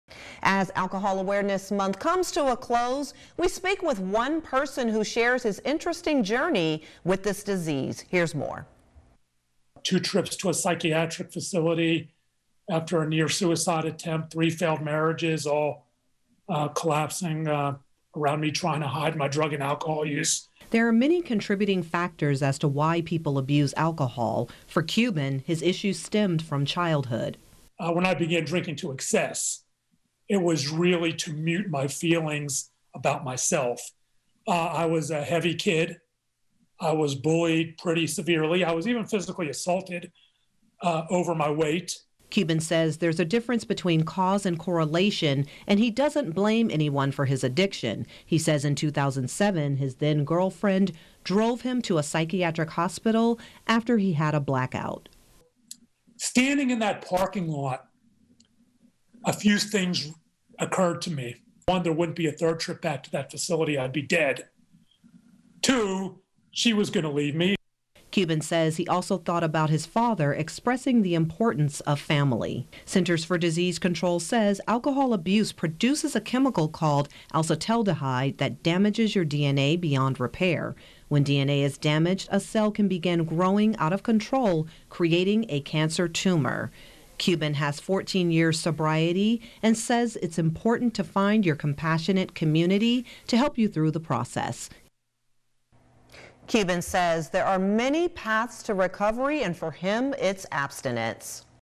Overcoming alcohol addiction
As Alcohol Awareness Month comes to a close we speak with one person who shares his interesting journey with this disease.